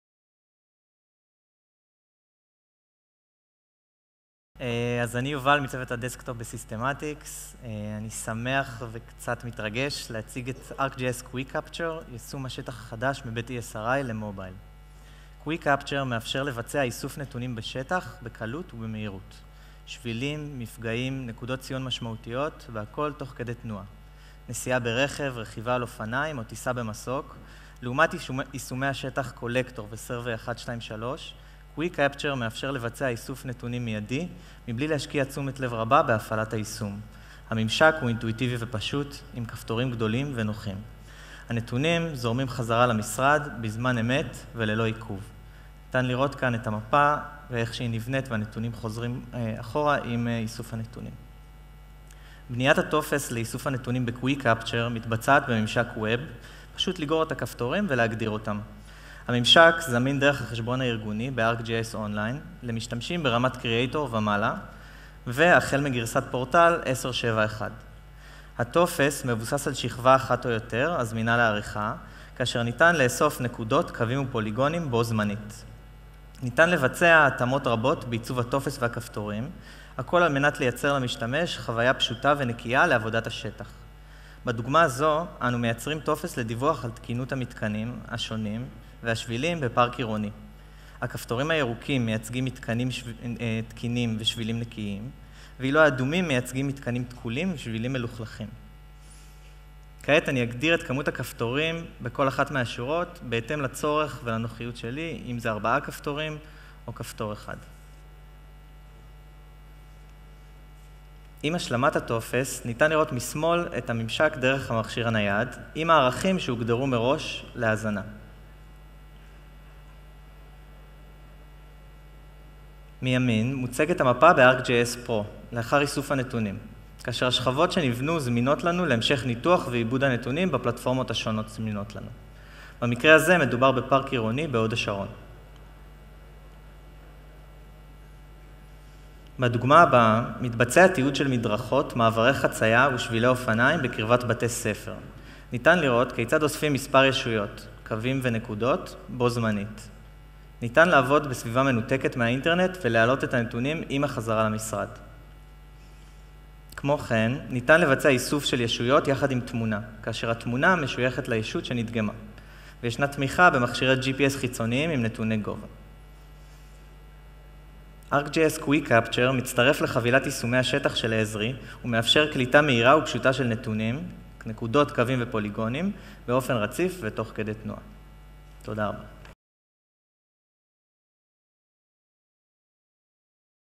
8. ArcGIS QuickCapture
יובל רינצלר מחברת סיסמטיקס בהרצאה שניתנה במסגרת כנס GIS לטכנולוכיית ESRI בנובמבר 2019